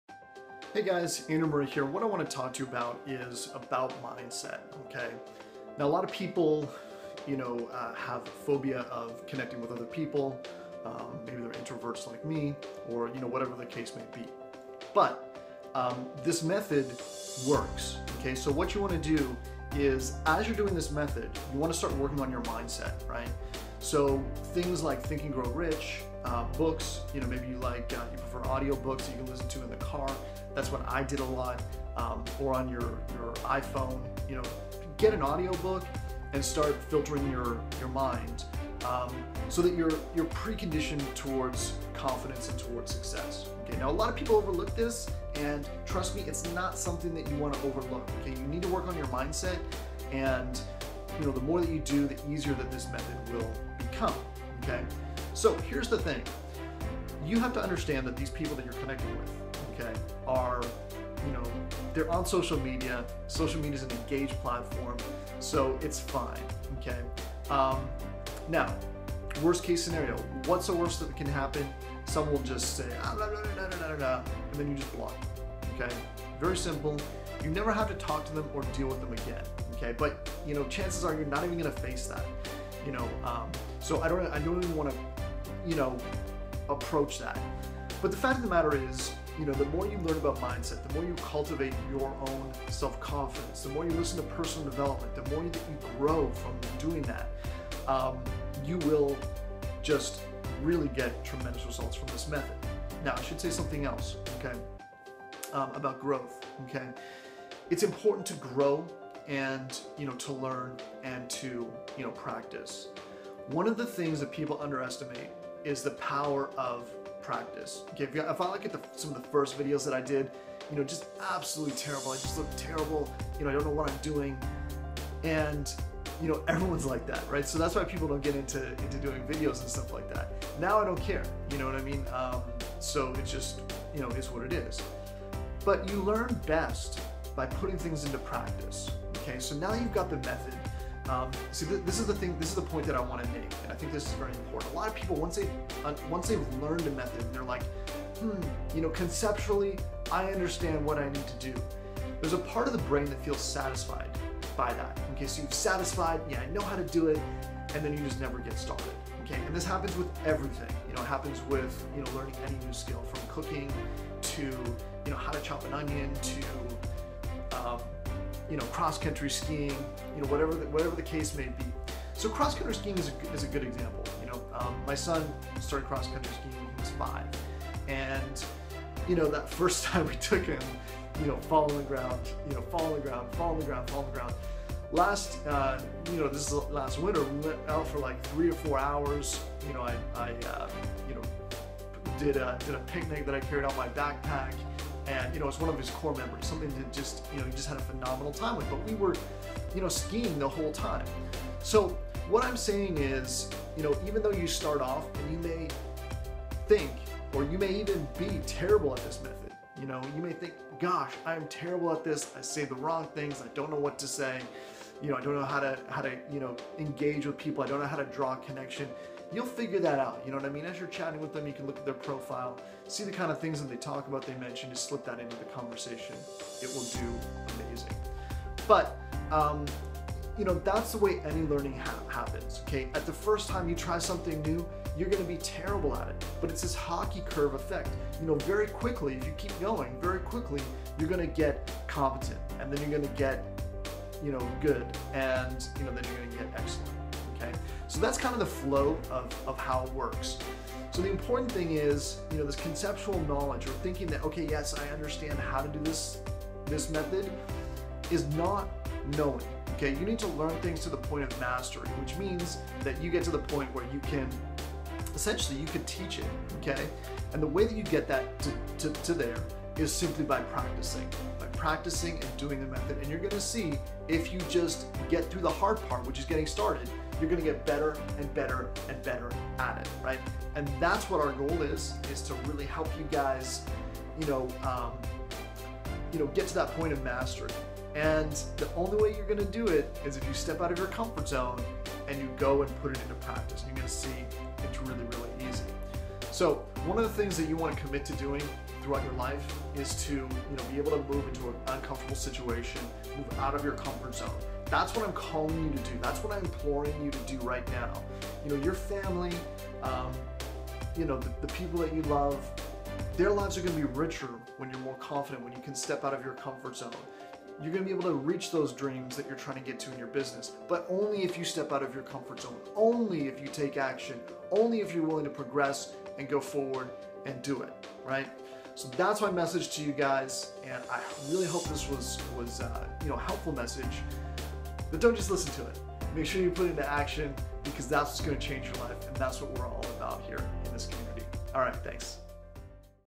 On Mindset
Mindset Video: What I've learned about mindset over the years.

Learn How To Run Highly Profitable YouTube Ads